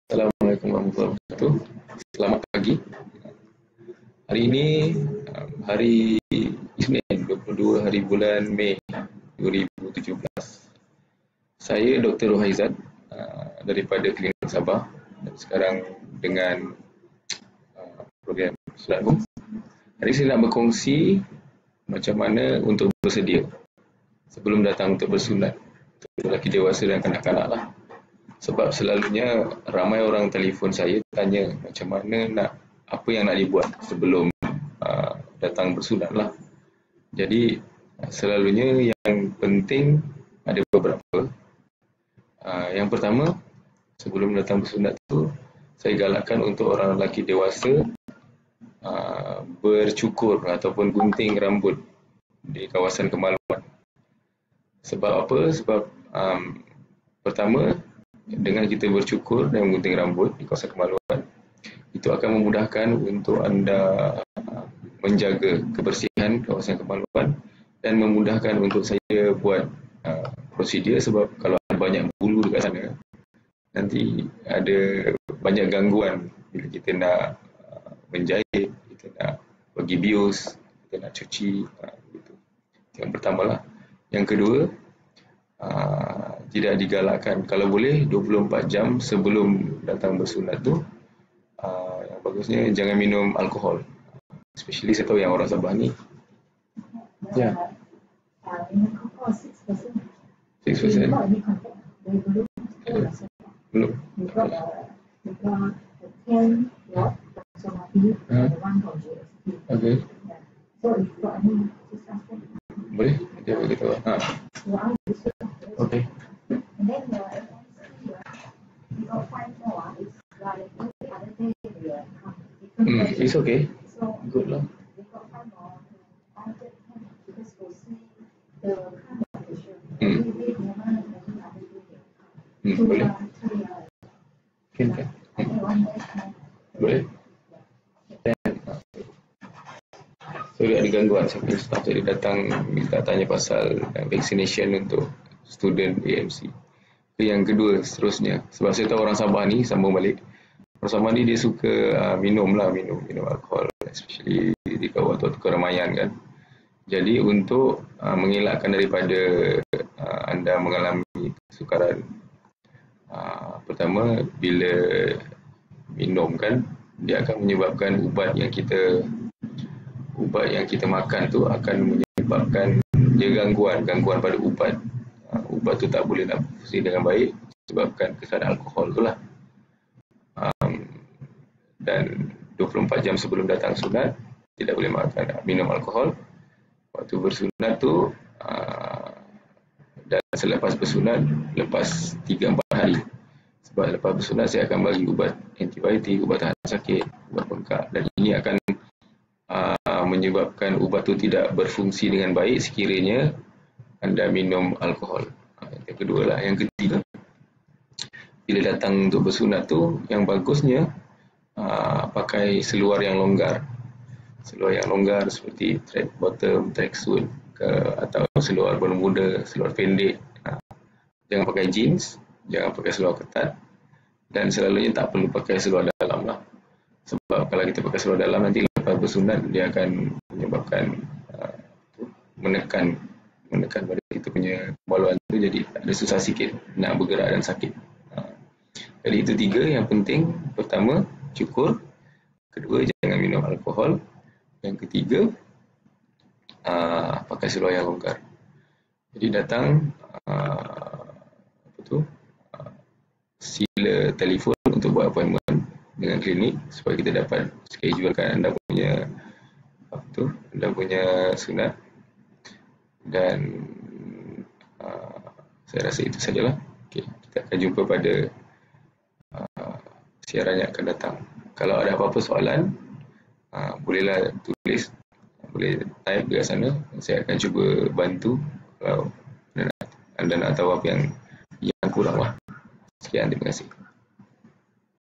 Persediaan sebelum bersunat/berkhatan untuk lelaki dewasa
3 Persediaan penting sebelum bersunat/berkhatan untuk lelaki dewasa:

1. Mencukur atau menggunting bulu di kawasan kemaluan. Ini akan memudahkan doktor untuk melakukan prosedur sunat dan juga akan memudahkan untuk anda menjaga luka sewaktu tempoh  penyembuhan.

2. Jangan minum alkohol sekurang-kurangnya 24 jam sebelum bersunat/berkhatan. Ini kerana kandungan alkohol di dalam darah akan mengganggu penyerapan dan keberkesanan ubat tahan sakit dan antibiotik yang diberi oleh doktor.

3. Pakai seluar yang longgar apabila datang untuk bersunat, seperti seluar trek atau seluar bermuda. Ini adalah untuk keselesaan anda bergerak selepas bersunat, kerana seluar yang ketat akan menyebabkan luka sunat tertekan.

SunatHome2u.com
Kepakaran kami adalah membantu kaum lelaki dewasa dan kanak-kanak untuk bersunat/berkhatan dengan cepat dan selesa, bagi mendapatkan "burung" yang sihat, bersih, yakin dan mengancam.